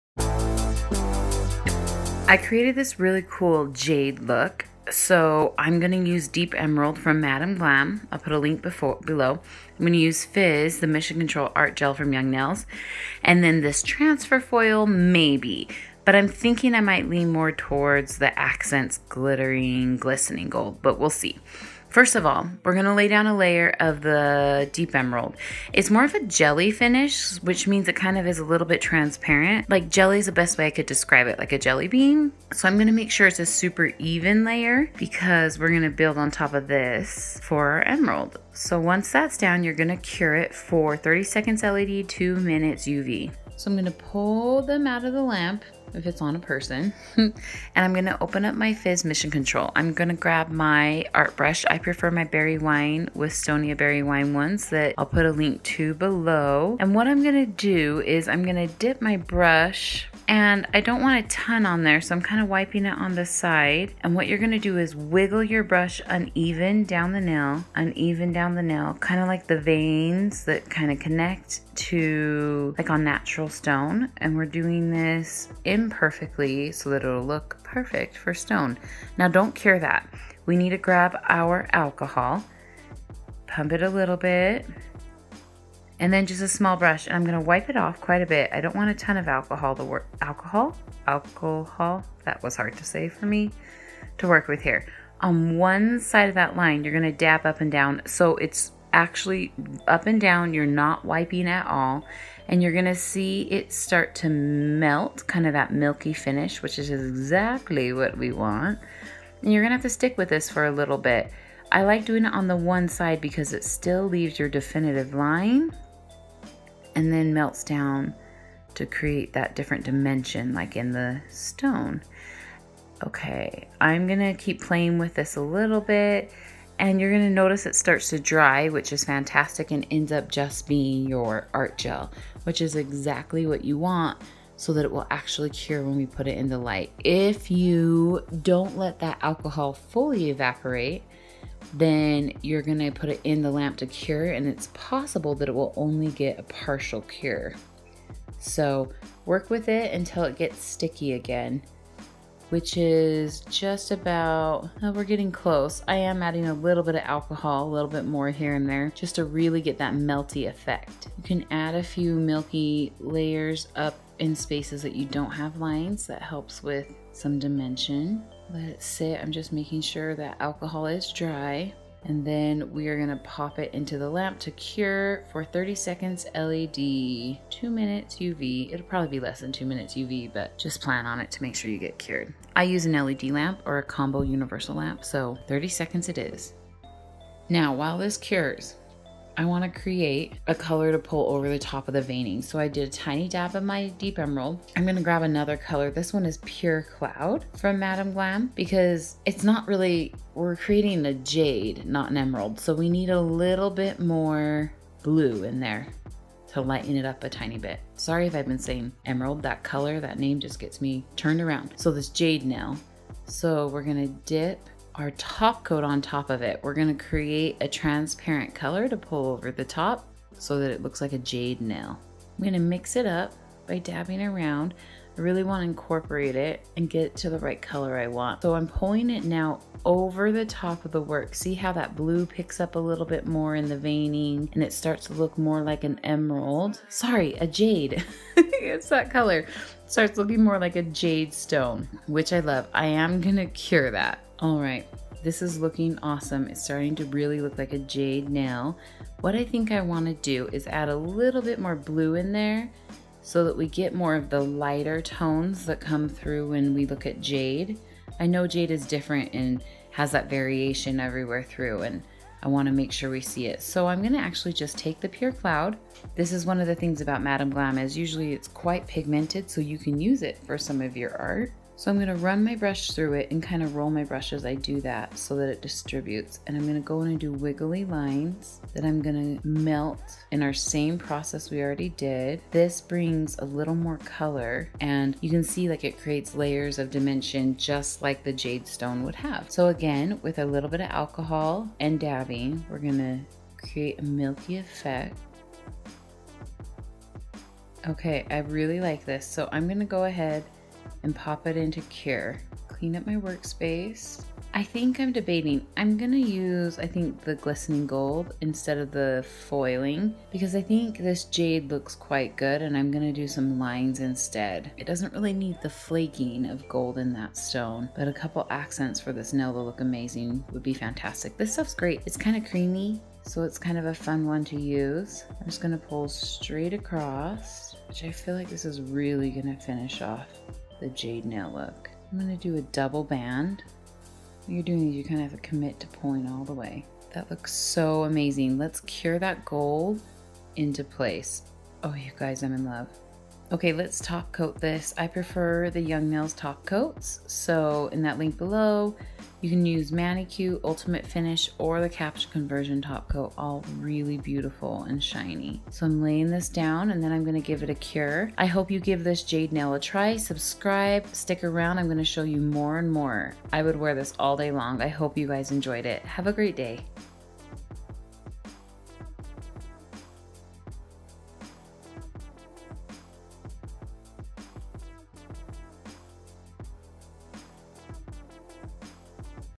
JADE NAIL ART | HILARY DAWN HERRERA
I love the faux stone and precious gems trend that I'm seeing EVERYWHERE right now! Follow along as I show you how to recreate this jade stone look for your clients that they are guaranteed to love!

PRODUCTS USED:

Mission control flex
Young Nails clear top coat

Winstonia Nail Art Brushes

Akzentz Gel Play Glisten Gold

I share tip and tricks from over 14 years experience in the nail tech industry and encourage you to check in daily!